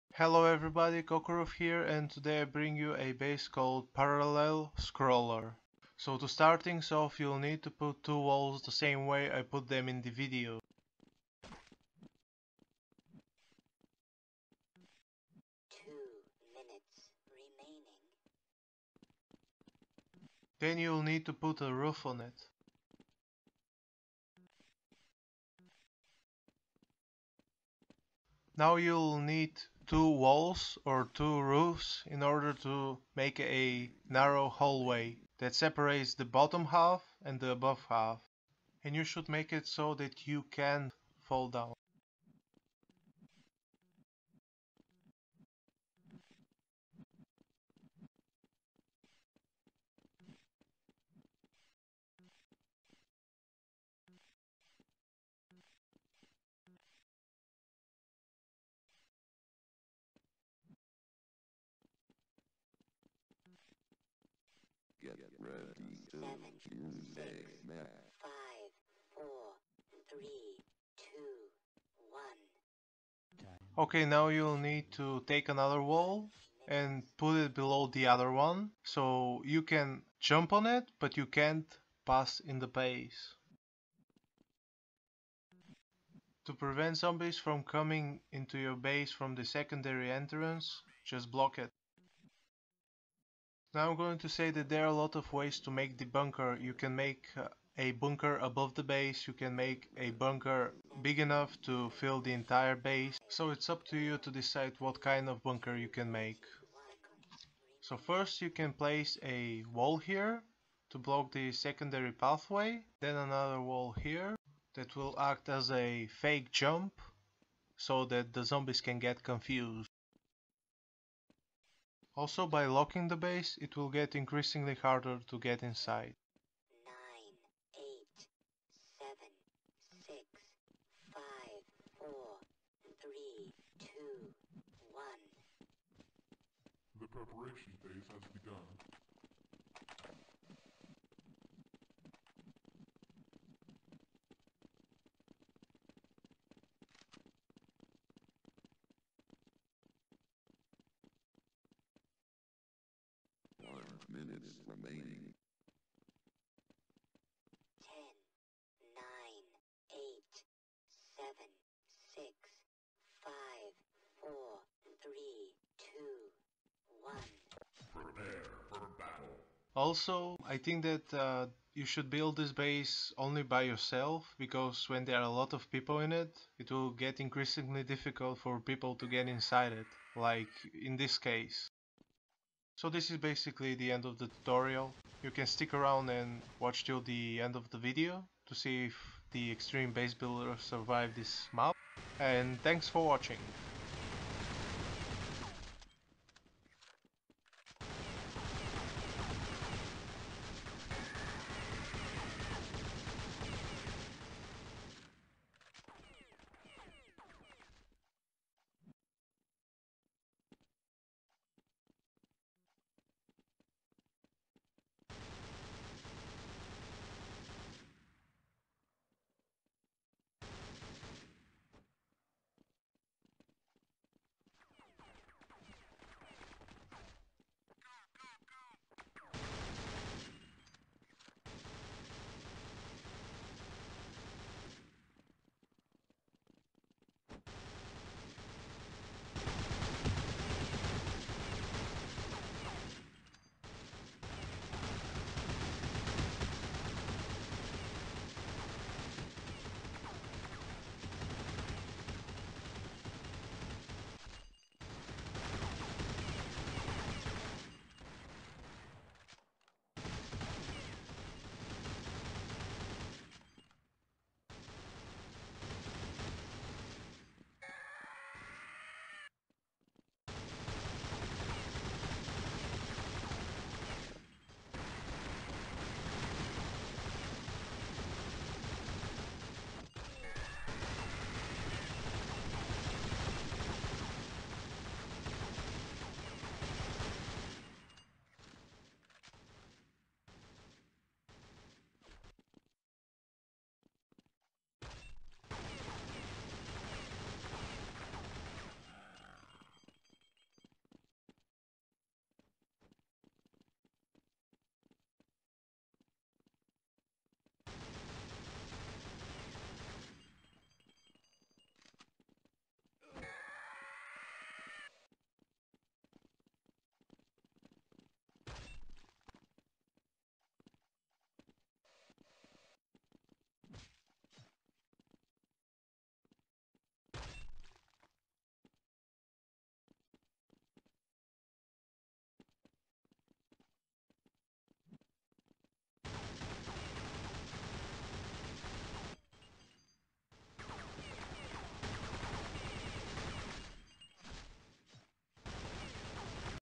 Parallel Scroller Tutorial [Base Builder] (+Commentary)
In this video tutorial I explain how I make the parallel scoller. Also I made a video on surviving in this base here is a